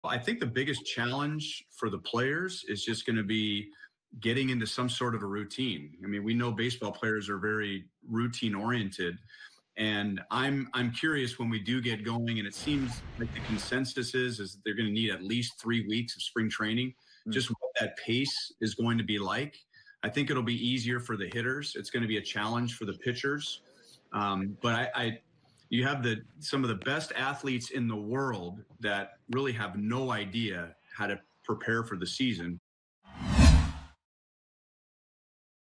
Ryan Lefebvre on challenges for players whenever baseball resumes
Ryan Lefebvre on challenges players will face whenever baseball resumes: "I think the biggest challenge for the players will be getting into some kind of a routine." Grillin' the AL Central